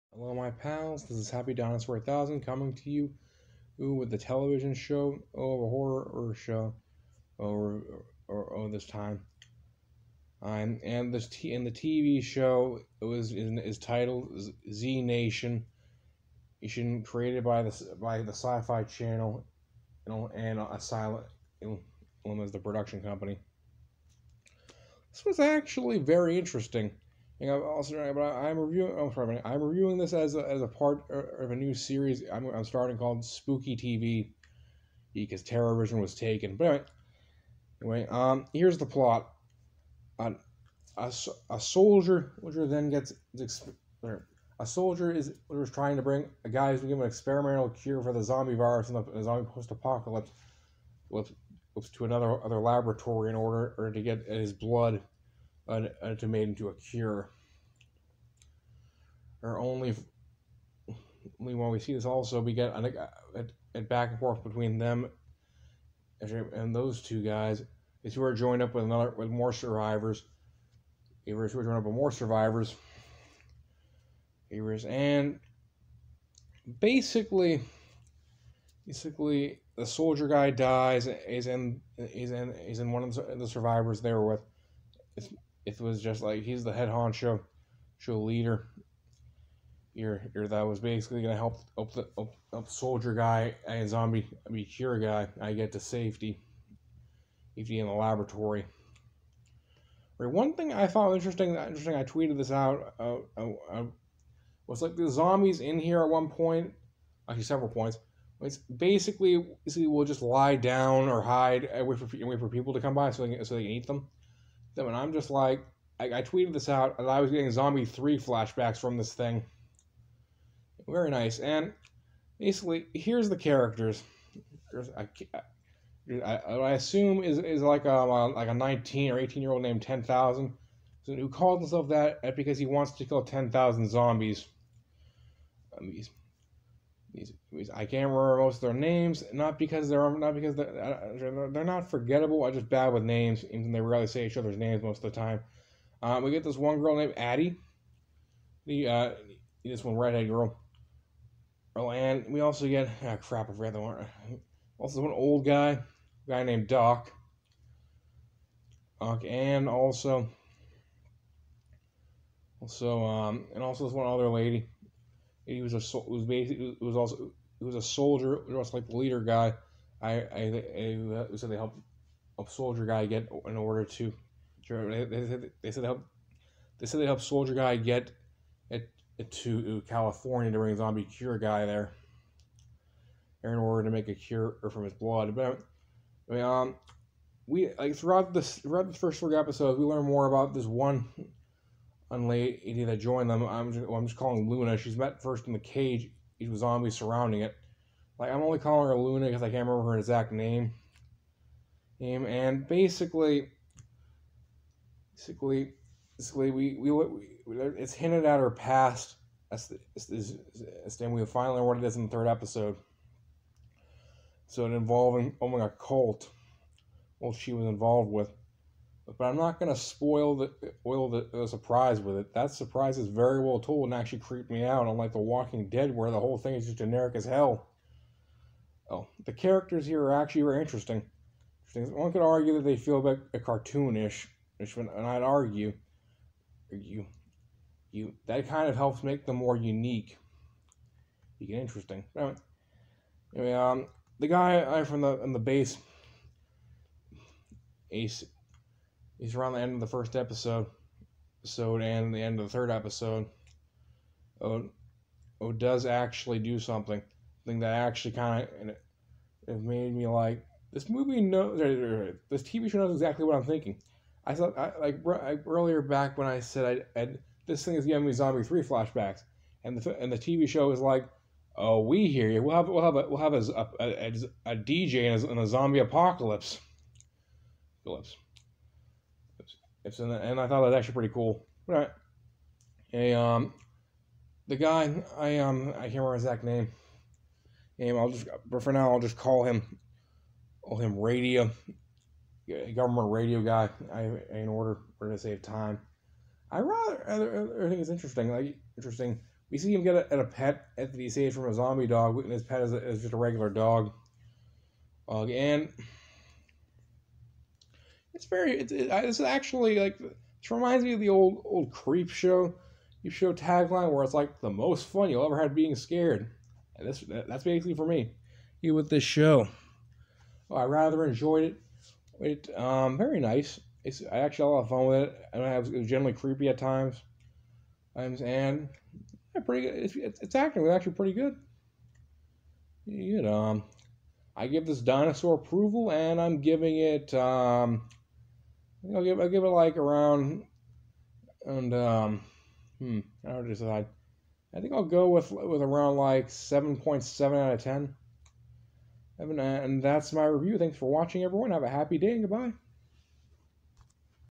Spooky TV! Part One: Z Nation
I review the first 3 episodes of Z Nation from the first season